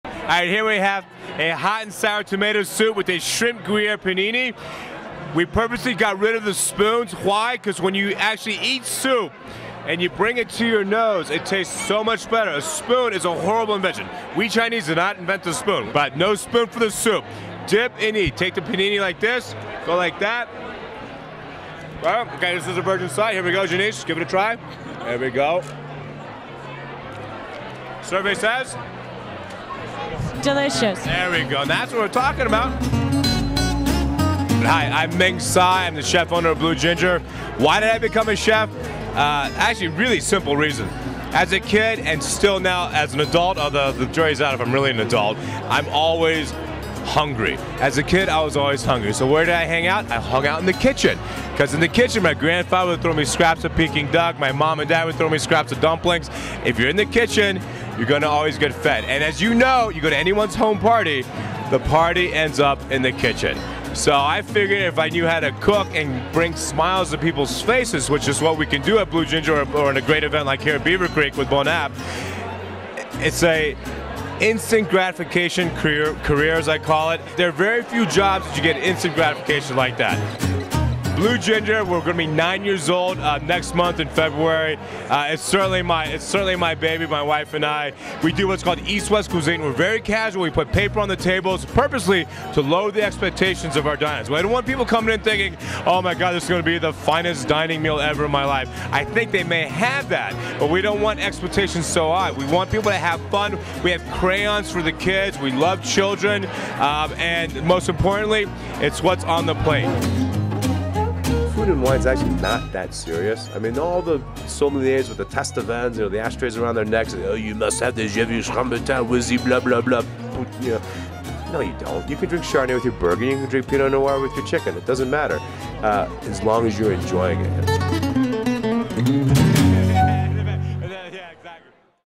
Ming Tsai
At the 2006 Beaver Creek Bon Appetit Culinary and Wine Focus, Plum was lucky enough to meet up with world famous chef and restaurant owner Ming Tsai.